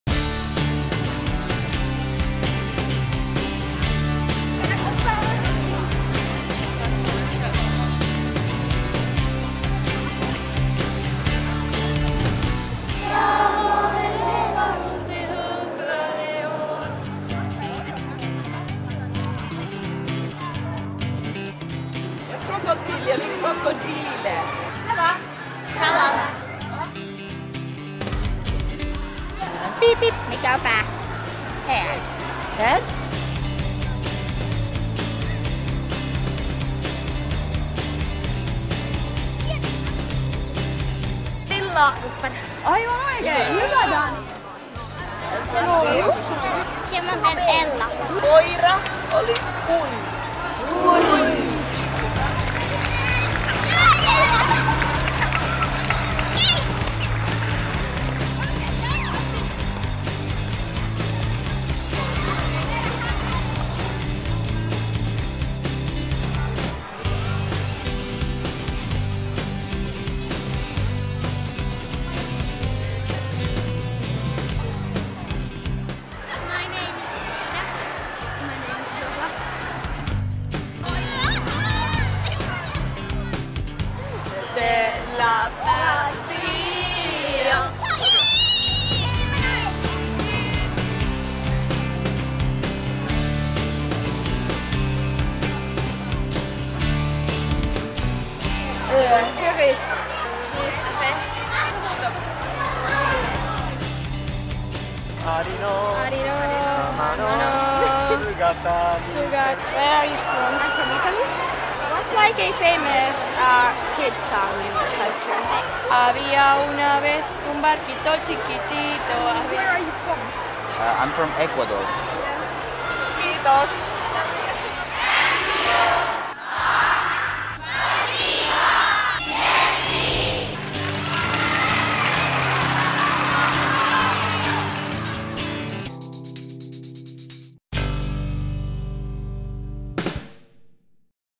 Kielikarnevaali 2019
Turun Kielivirta-hankkeen Kielikarnevaali yli 1000 Turun, Naantalin ja Raision alakoulujen kieliluokkien oppilaille. Kielikarnevaalissa tutustuttiin vieraisiin kieliin toiminnan, leikin ja pelaamisen avulla.